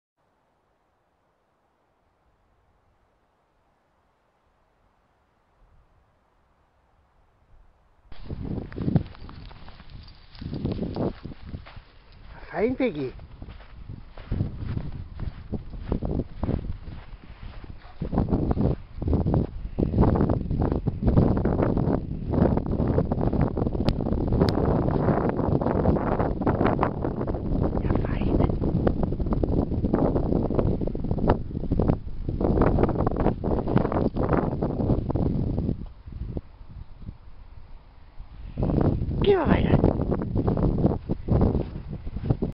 Unsere Peggy im Schnee 😊🐶🌾🌳🌨❄️☃️⛄️🌞
Königsbach-Stein, den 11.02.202: gemeinsam laufen mein Treuer Begleiter & Persönlicher Fitnesstrainer Peggy und ich bei sonnigen -3°C und strahlend blauem Himmel auf einem schönen langen Spaziergang durch die schneebedeckten Felder auf dem Königsbacher Berg 👍🏻💪🏻😊🐶🌾🌳🌨❄️☃️⛄️🌞